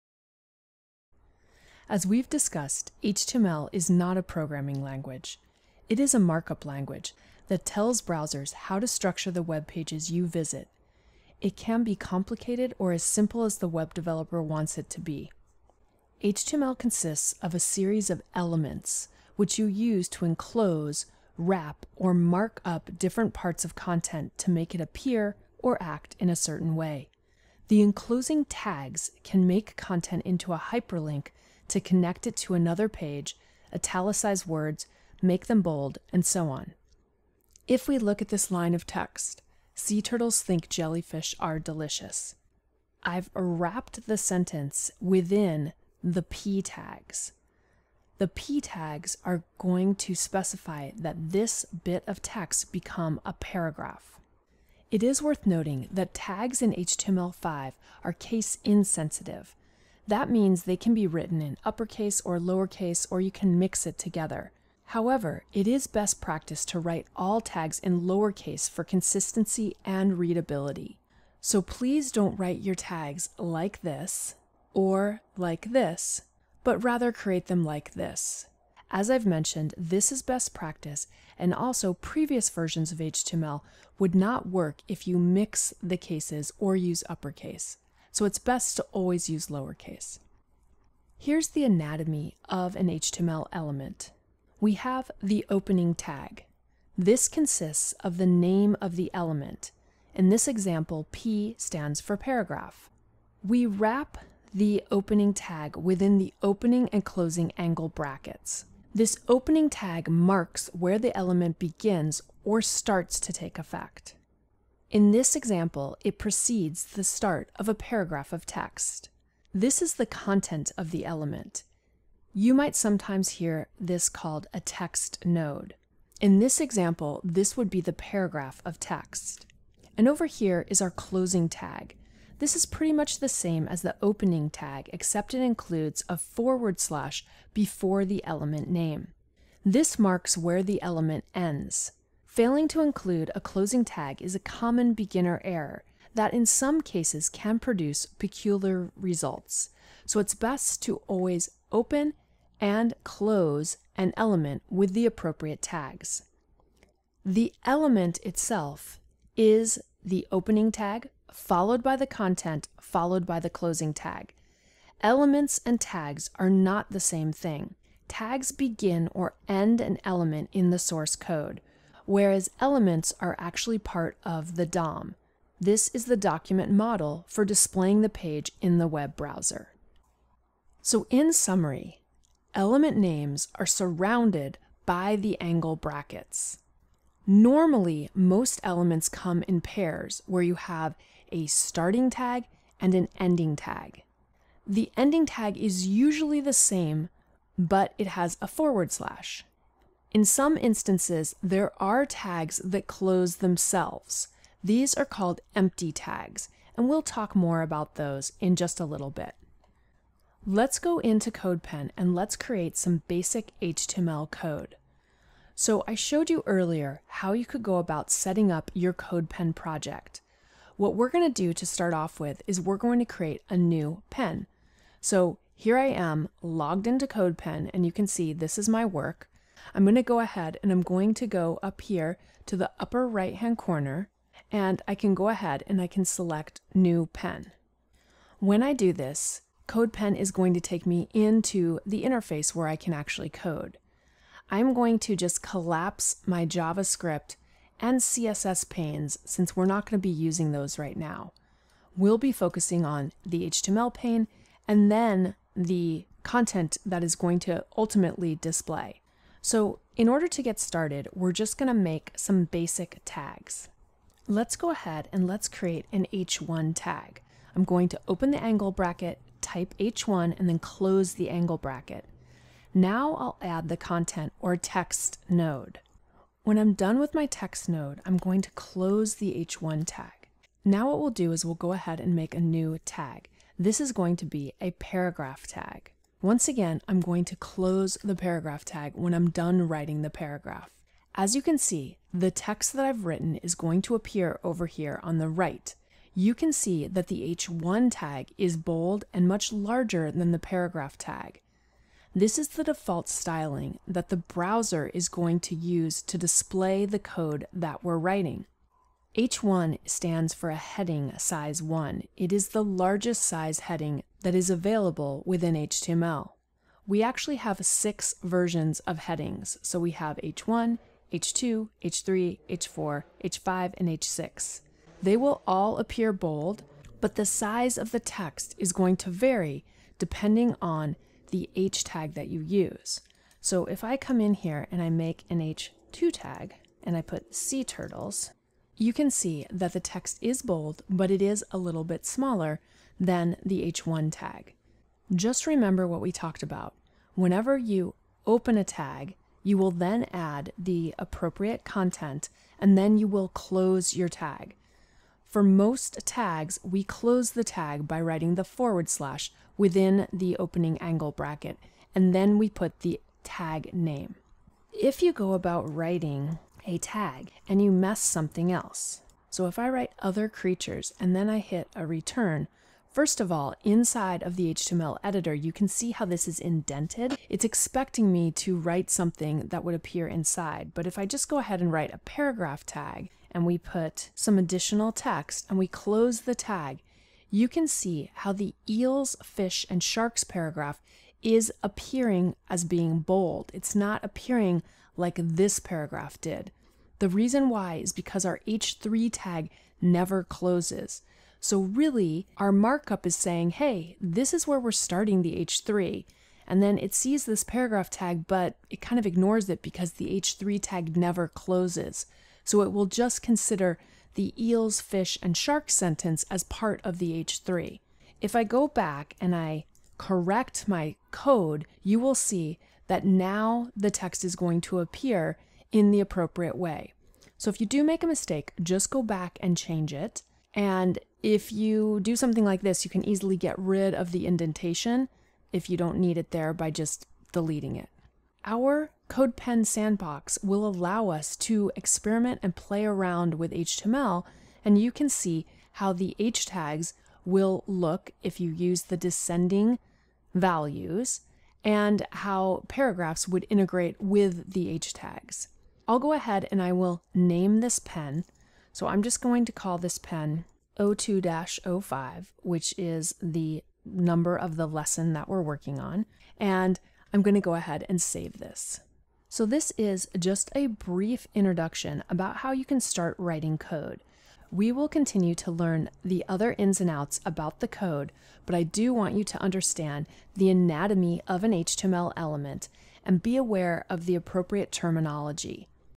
Writing HTML (05)
Learn the basics of writing HTML, I'll teach you some important terminology and get you up and coding. You will learn the basic semantics of this web development language.